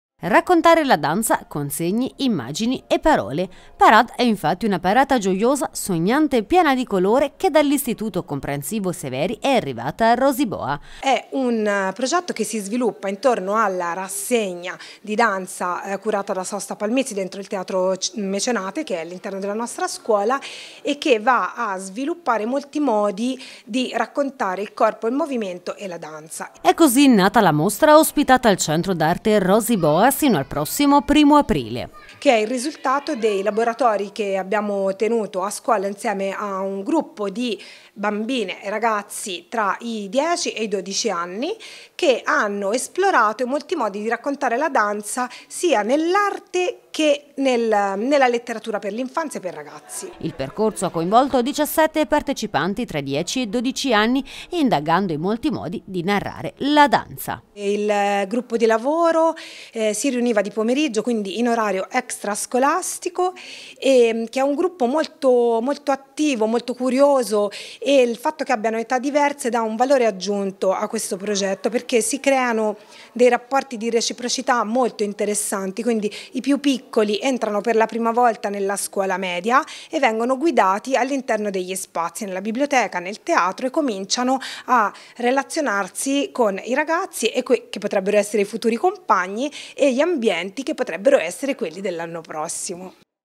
Parade, la mostra degli studenti del'I.C. Vasari al centro d’arte Rosy Boa
Raccontare la danza con segni, immagini, parole.
Parade è una parata gioiosa, sognante e piena di colore dall’I.C. Severi è arrivata a Rosy Boa.
Il percorso ha coinvolto diciassette partecipanti tra i dieci e i dodici anni e indaga i molti modi di narrare la danza.